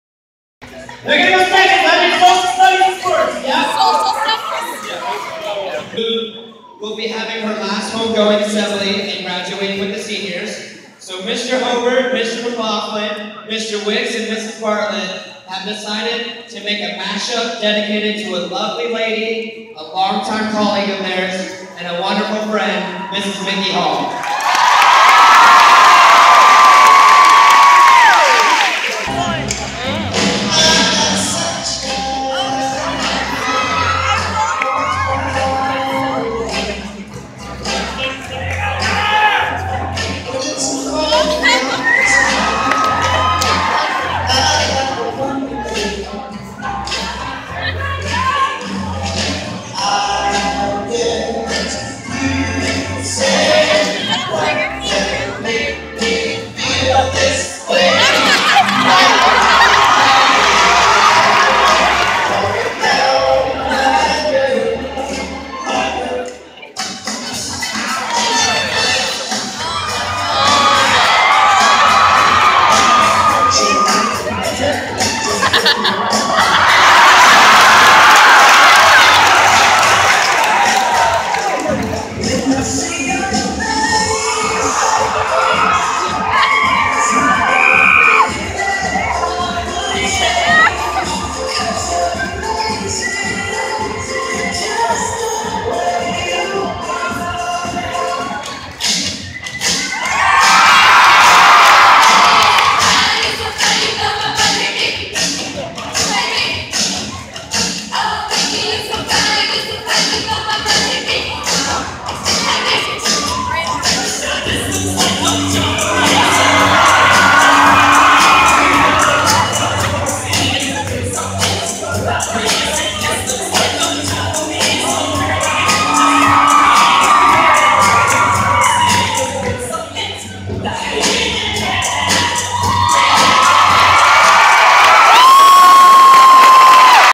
RHS home going assembly lip sync battle
Social studies teachers dedicate their songs to Micki Hall who is retiring this year